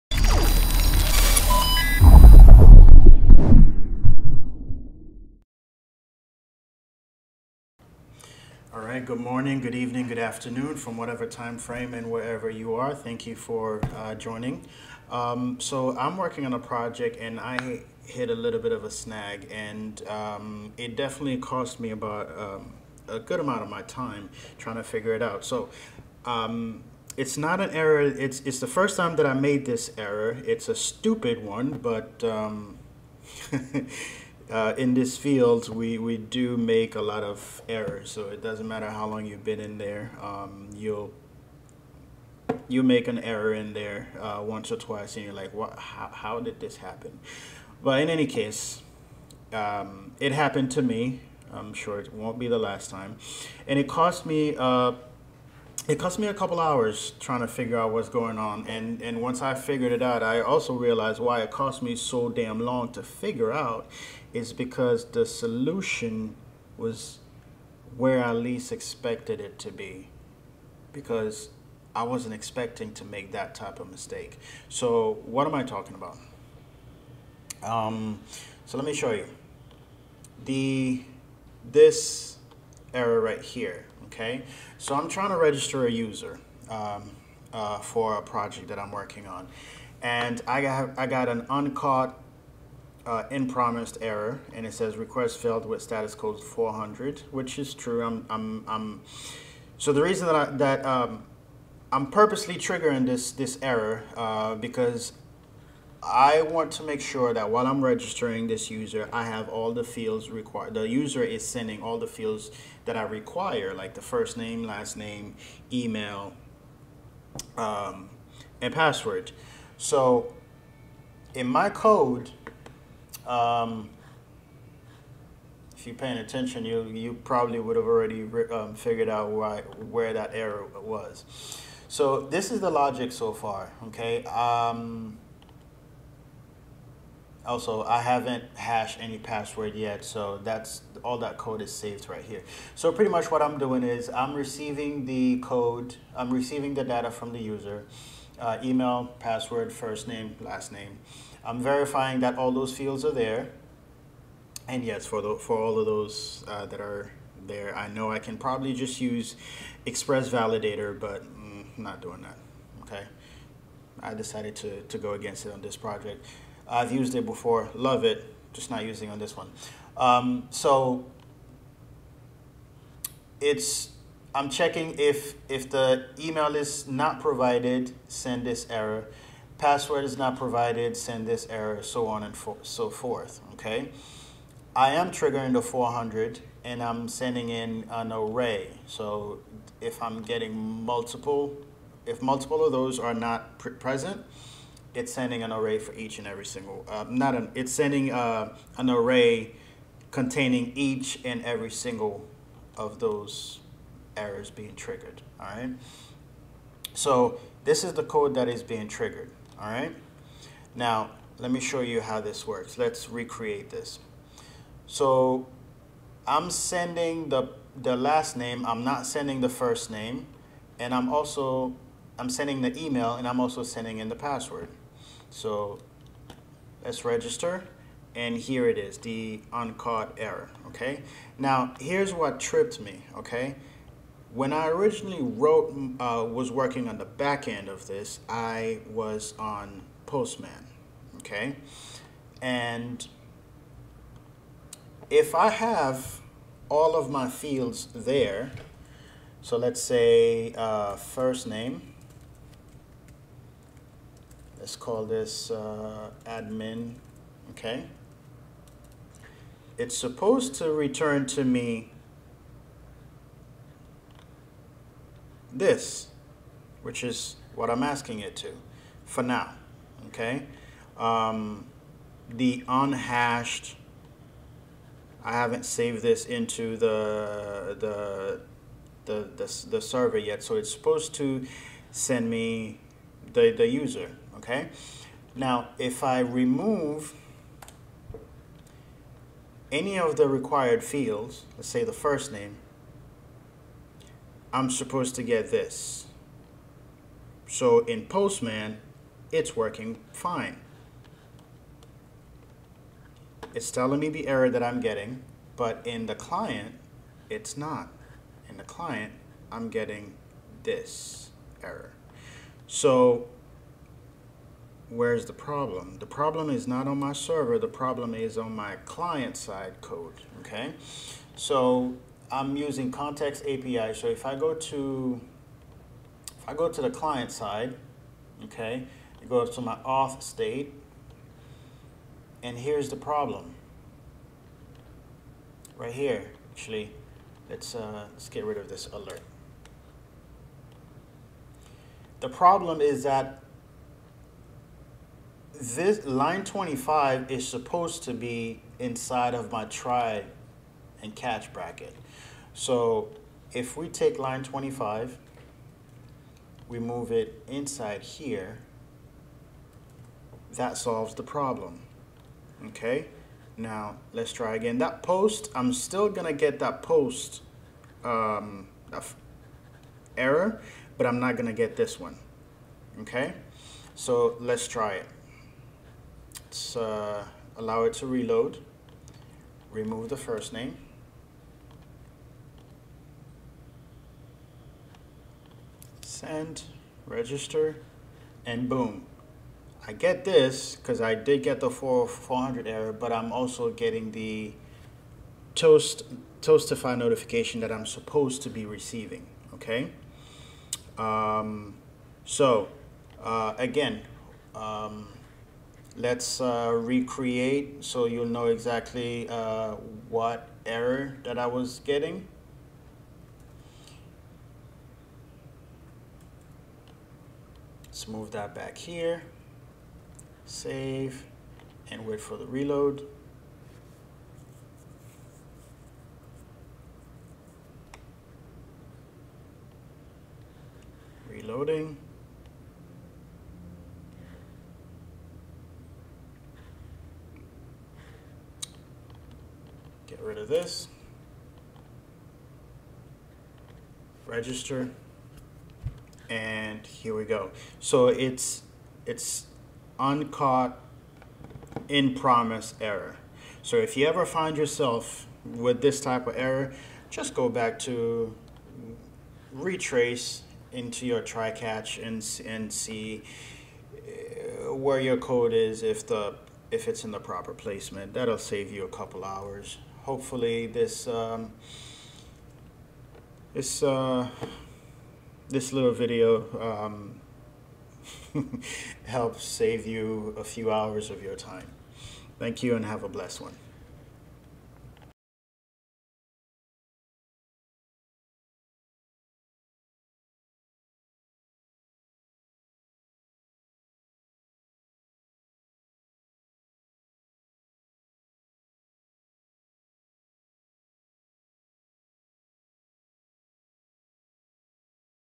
How to debug Error (In Promise) Uncaught
We are debugging Uncaught (in Promise) status 400 error in Axios.